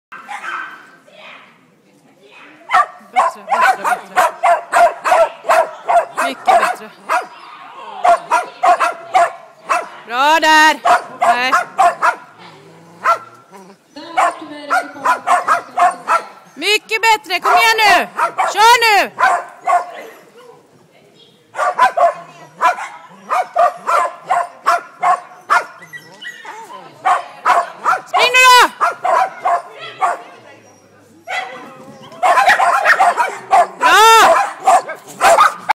Kungsör hopp january 2 2011
Much better jumping, but still not good. My bad after the weaves, should have been more distinct